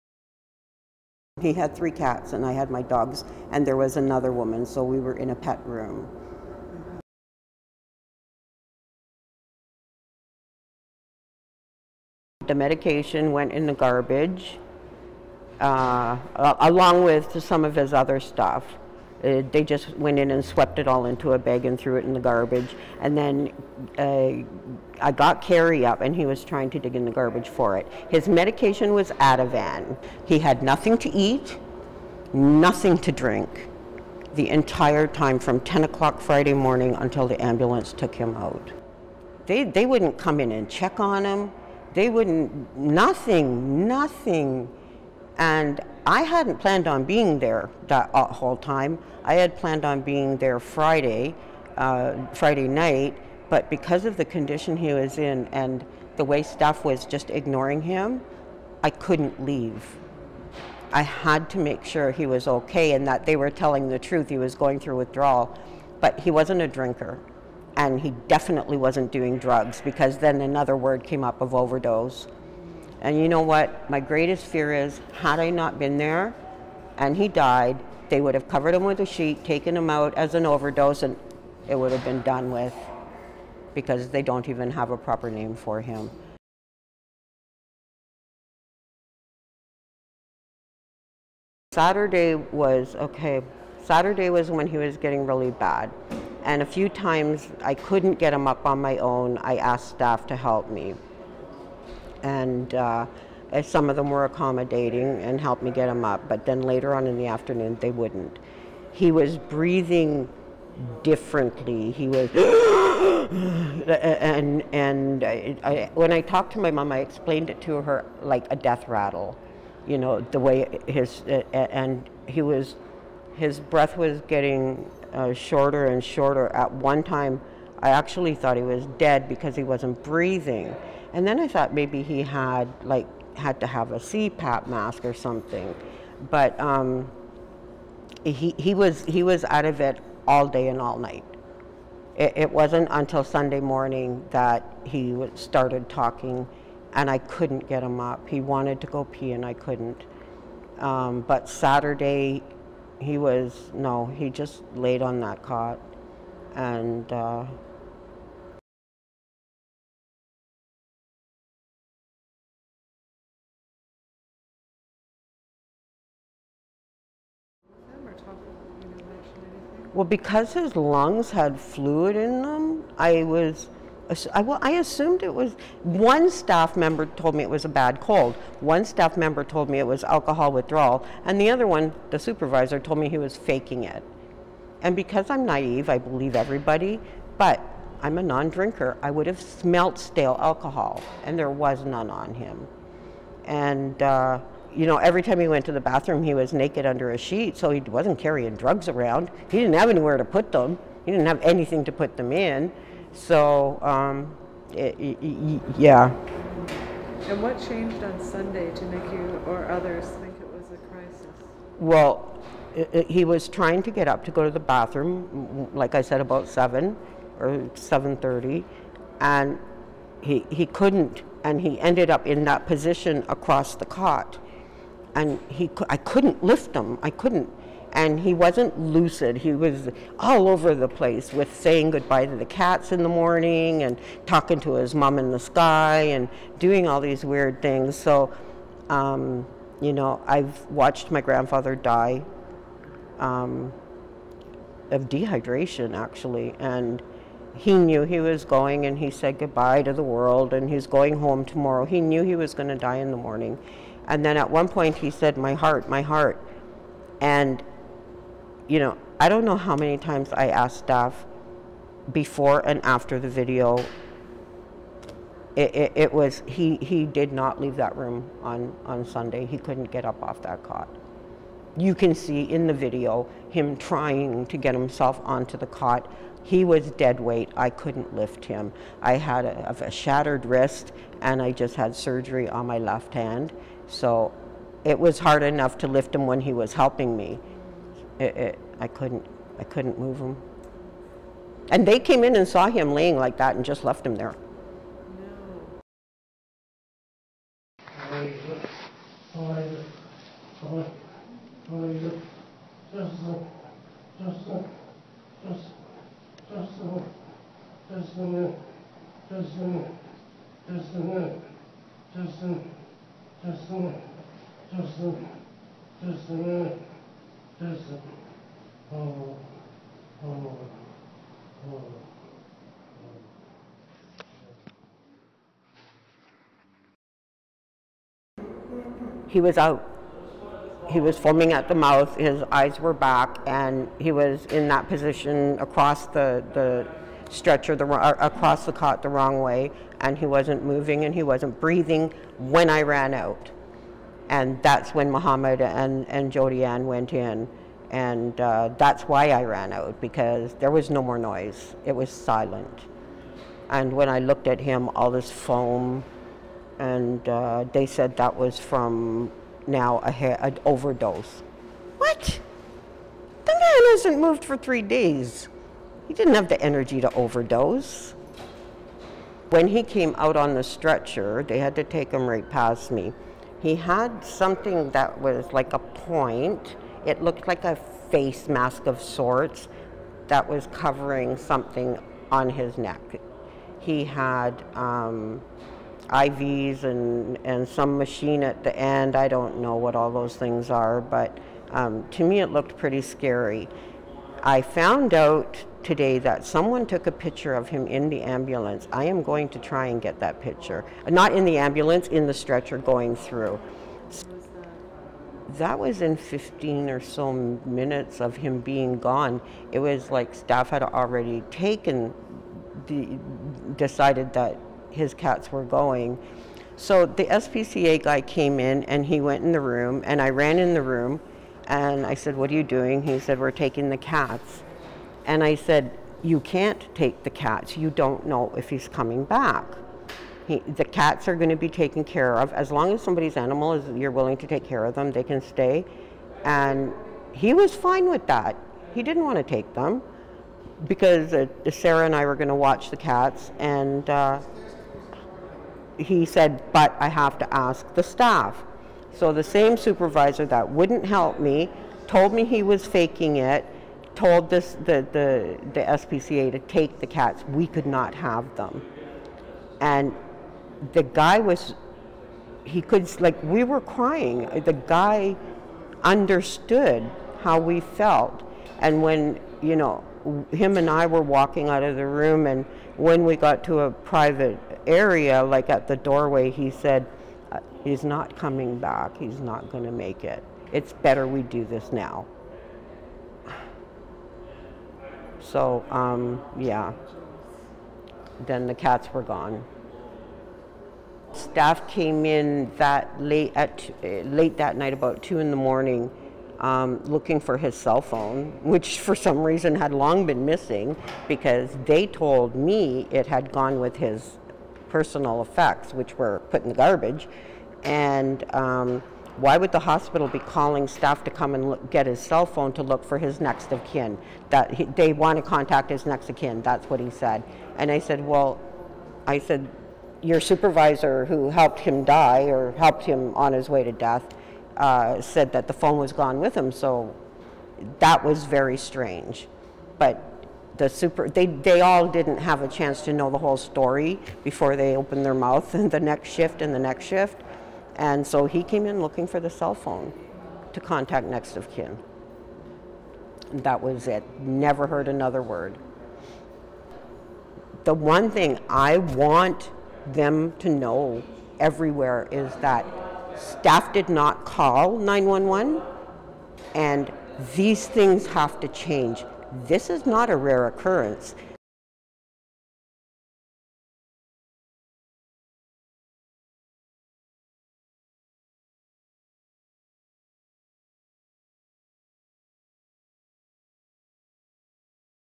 Witness Who Took Video Recounts Near Death of Homeless Man at Moss Park Armoury
Debi OKane speaks to documentary film maker Rebecca Garrett about the video she took of Carey, a man struggling to live and breathe in the pet friendly section of the Moss Park Armoury.

Carey was taken by ambulance from the Armoury on Sunday, January 21, 2018. We still cannot locate him. The Canadian Federal Government and City of Toronto opened the Armoury as a response from pressure by activists during Toronto's ongoing ShelterCrisisTO.